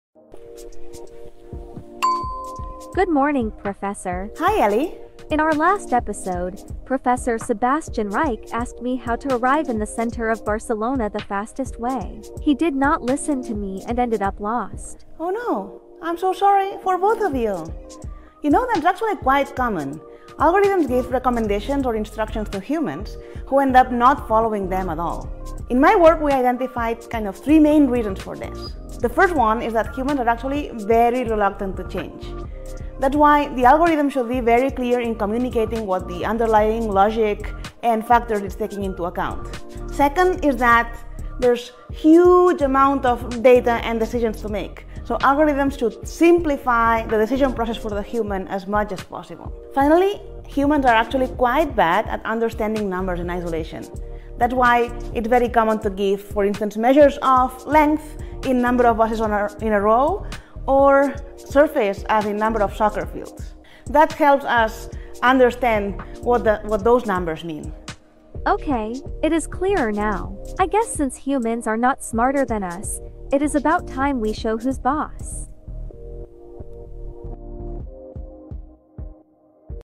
Are algorithms useful?
Technology has put a once-inconceivable amount of knowledge and practical advice at our fingertips. Why are humans often reluctant to trust it? Prof. Anna Sáez de Tejada Cuenca reveals three big reasons why we might not put our faith in algorithms in our latest ElevatorTips video.